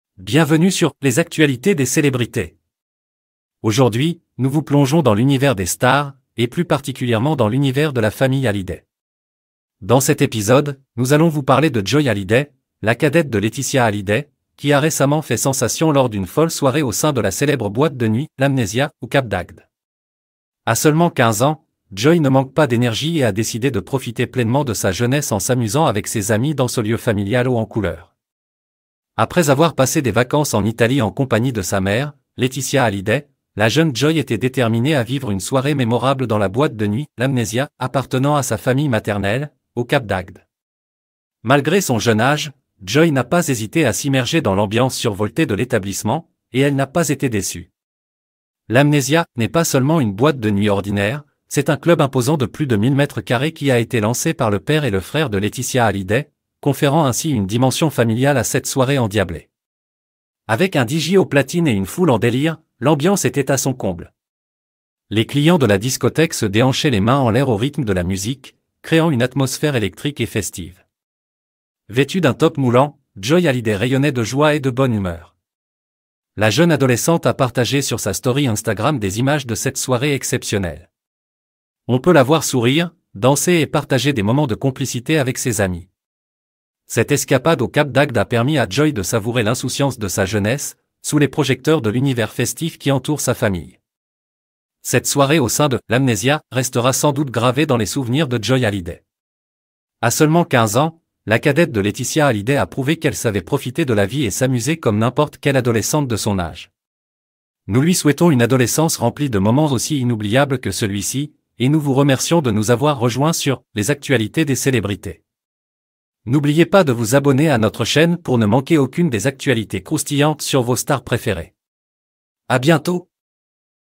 Joy Hallyday enflamme l'Amnesia : soirée sensationnelle !
Découvrez l'ambiance survoltée de Joy Hallyday à l'Amnesia au Cap d'Agde ! Une soirée inoubliable dans la boîte de nuit familiale.

1. "Joy Hallyday brille à l'Amnesia!"
2. "Soirée électrique avec Joy Hallyday!"
3. "Folle nuit au Cap d'Agde!"
4. "Laeticia's Daughter rocks the Amnesia!"
5. "Family Vibes: Joy's Memorable Night!"